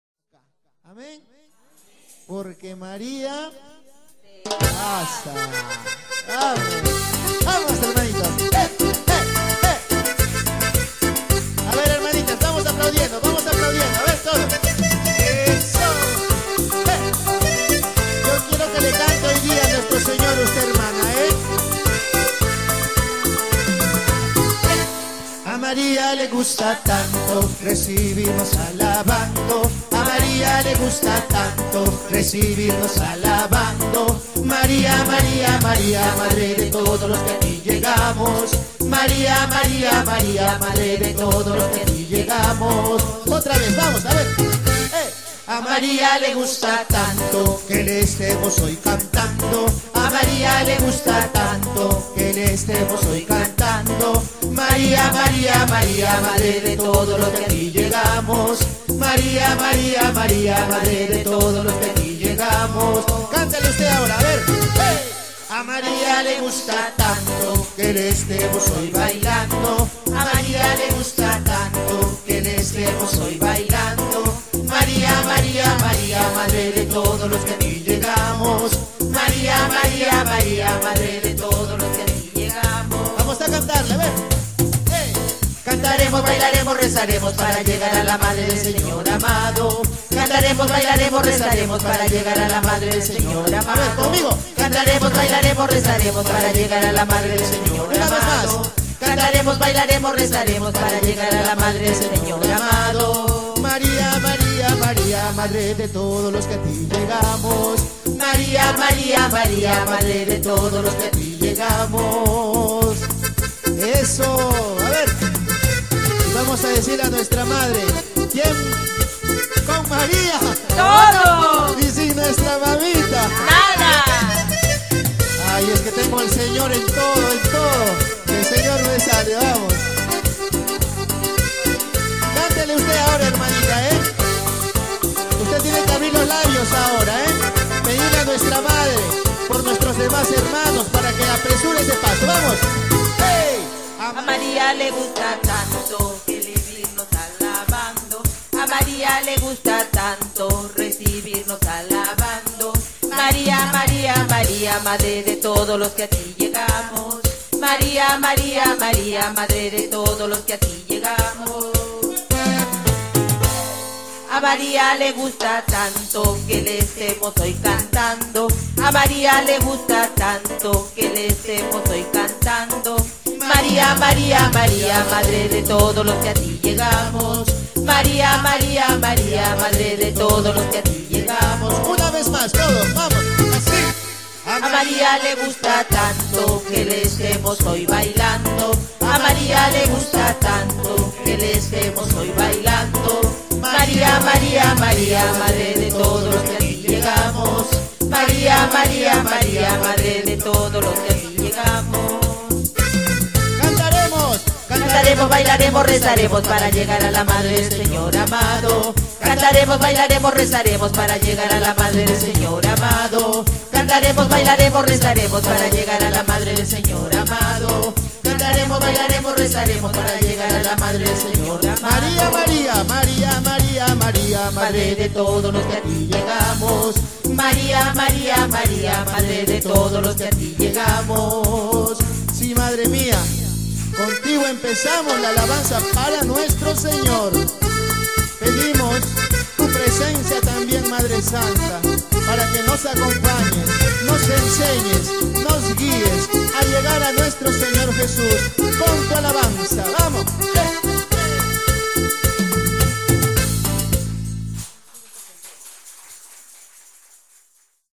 CANTO A LA VIRGEN MARÍA (SEMILLITAS DE JESÚS Y MARÍA-MOLLENDO)
ALABANZA EN VIVO DE HOY MIERCOLES 11 DE JUNIO 2014
NOS REUNIMOS TODOS LOS MIERCOLES DE 7PMA  10 PM PARA ALABAR AL SEÑOR CALLE FCO BOLOGNESI 511 ALTO INCLAN